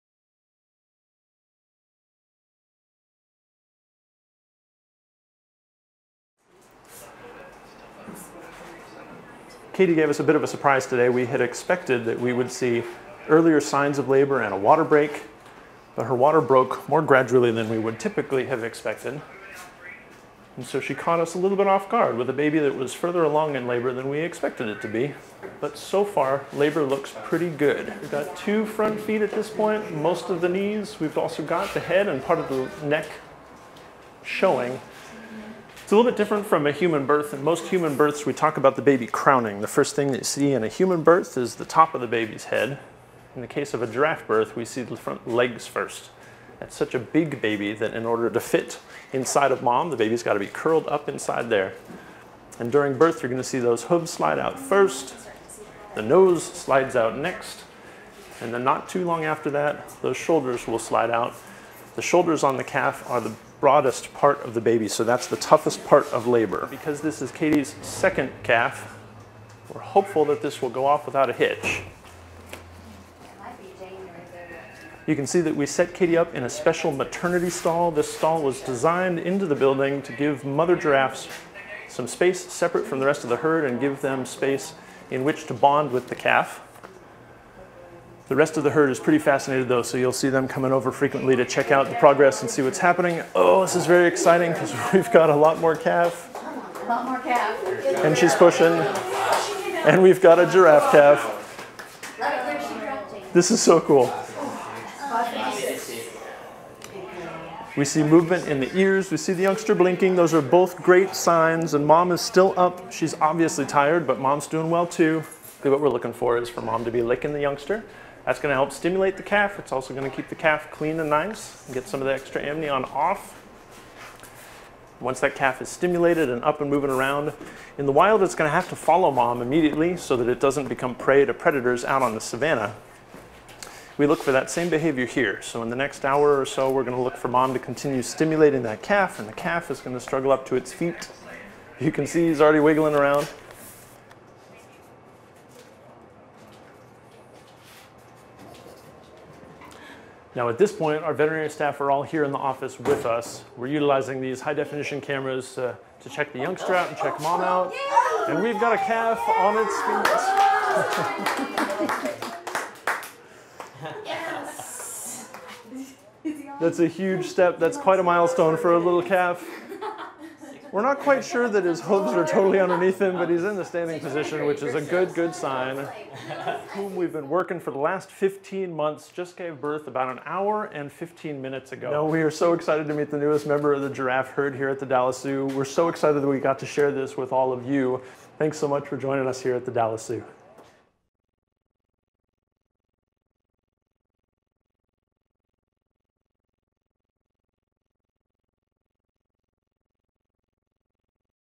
Katie the Giraffe Gives Birth!
Katie the Giraffe gave birth! Watch the highlights as captured on Animal Planet's Live Cam.

More APL!VE live streams!